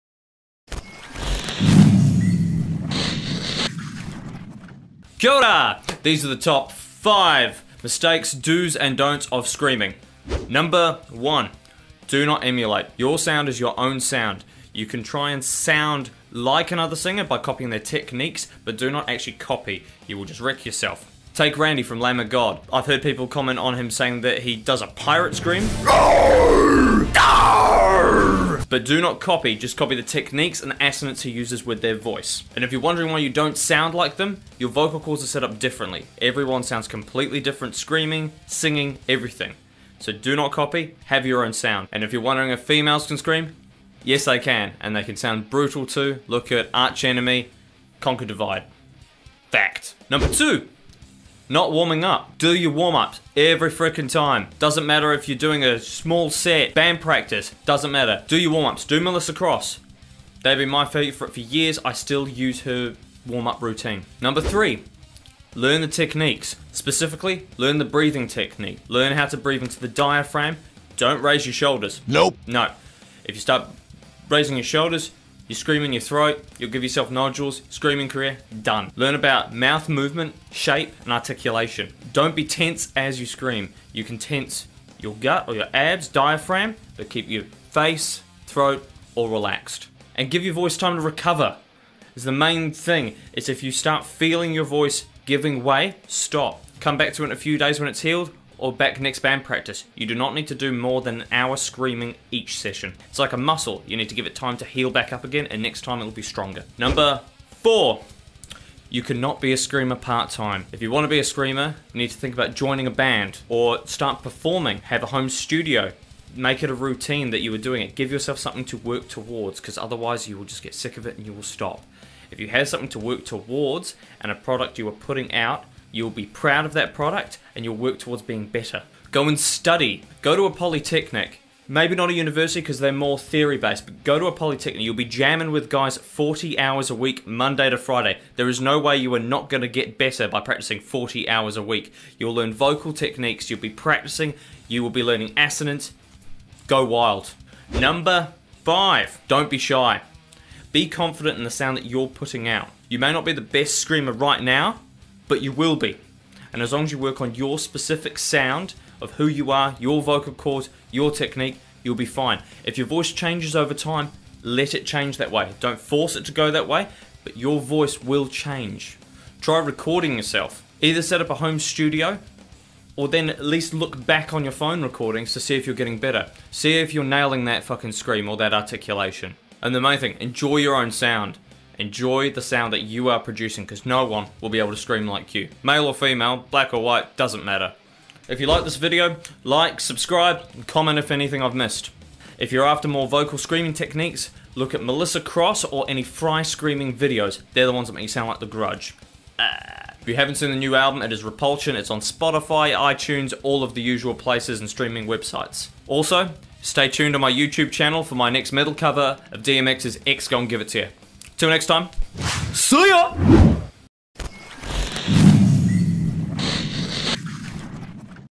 Top 5 Mistakes/ Do's and Don't of Screaming
Learn some simple tips, tricks and Mistakes of learning to scream